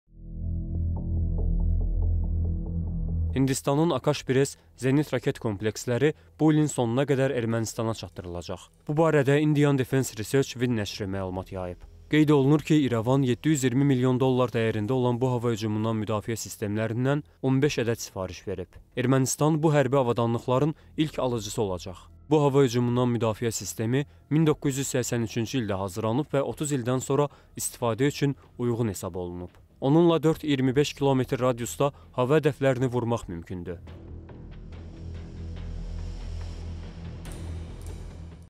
Ermənistan bu ölkə tərəfindən yeni silahlarla təmin olunacaq
Hindistanın "Akash-1S" zenit-raket kompleksləri bu ilin sonuna qədər Ermənistana çatdırılacaq.
Bu barədə "Indian Defence Research Wing" nəşri məlumat yayıb.
Qeyd olunur ki, İrəvan 720 milyon dollar dəyərində olan bu hava hücumundan müdafiə sistemlərindən 15 ədəd sifariş verib. Ermənistan bu hərbi avadanlıqların ilk alıcısı olacaq.
Bu hava hücumundan müdafiə sistemi 1983-cü ildə hazırlanıb və 30 ildən sonra istifadə üçün uyğun hesab olunub. Onunla 4-25 kilometr radiusda hava hədəflərini vurmaq mümkündür.